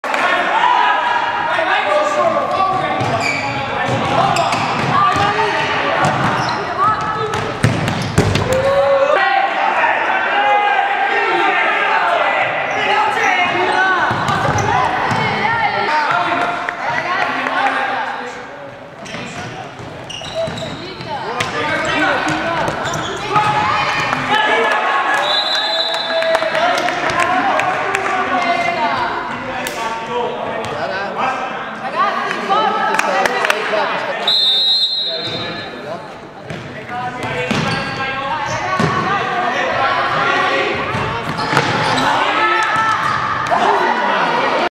06052016.Occasione e parata Real Fradeo Dt Color, occasione e palo Ischia, gol Alessio 2-6
Ischia - Real Fradeo Dt Color 2 - 6